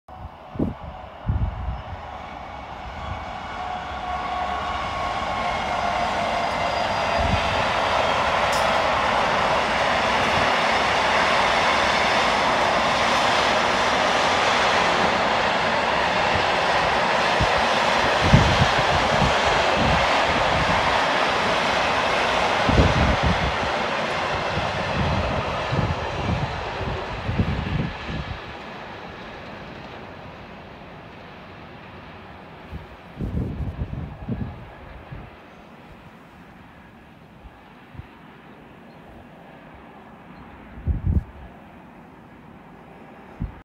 70814 on the Log Train 30/10/18
While waiting for the Dalesman the Colas Rail Carlisle Yard to Chirk Kronospan log train appears apparently on a new earlier path, the 70 was seen coasting downgrade at Helwith Bridge